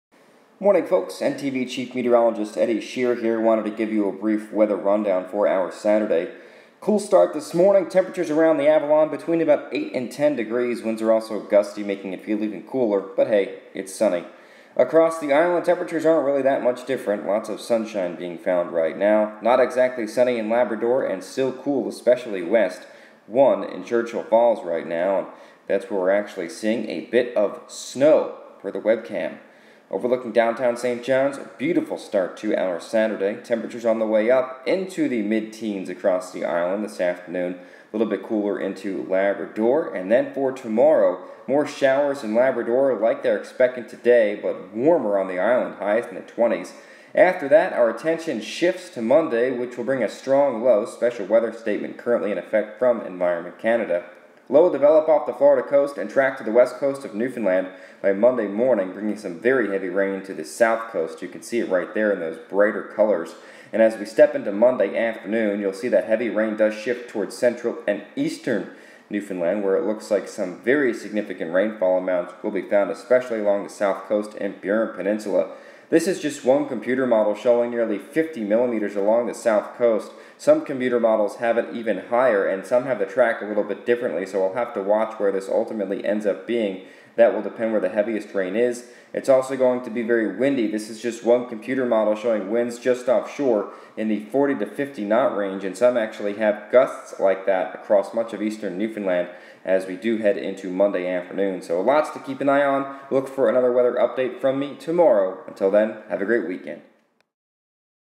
NLwx Briefing For Saturday, September 20, 2014
Quiet weekend, but tracking the storm for Monday